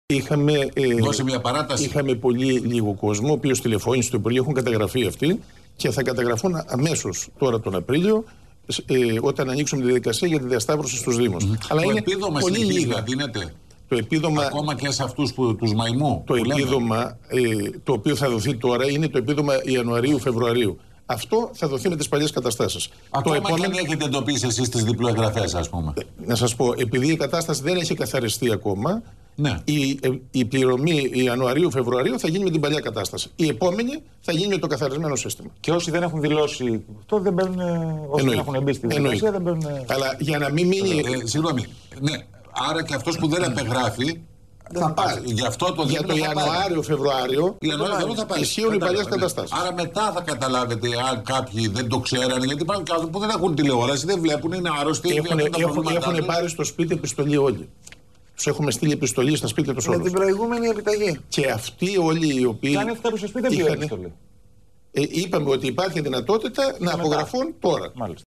NewsIt.gr: Οι μαϊμού δικαιούχοι θα πάρουν ξανά το επίδομα
Βίντεο από την πρωινή εκπομπή του Mega